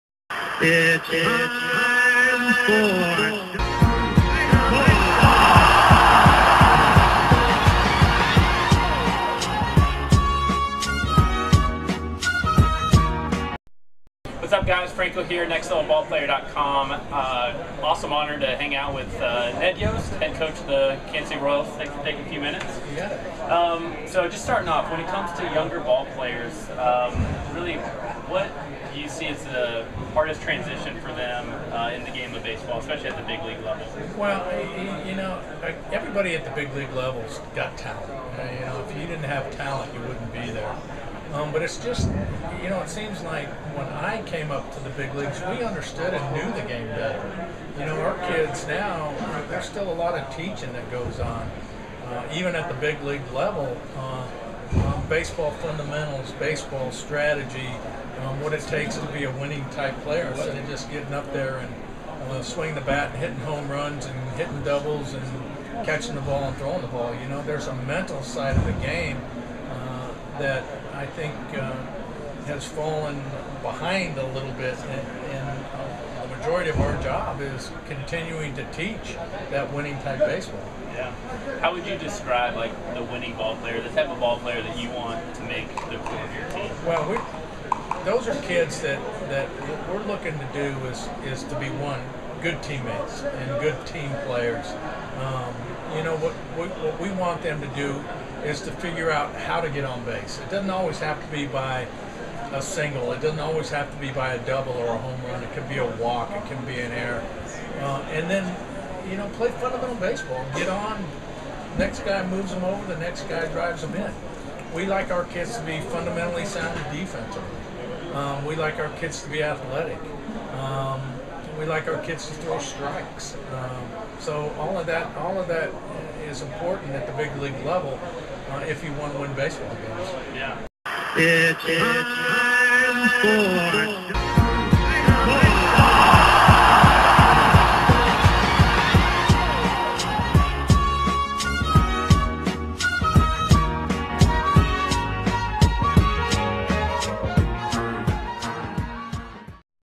Royals Ned Yost's Challenge to Serious Ballplayers
Ned Yost sits down with Franco from Next Level Ballplayer to discuss the biggest adjustment for ballplayers at the MLB level, how he describes a winning ballplayer he wants on his team and more.

and sign-up to the newsletter to receive baseball interviews right to your inbox.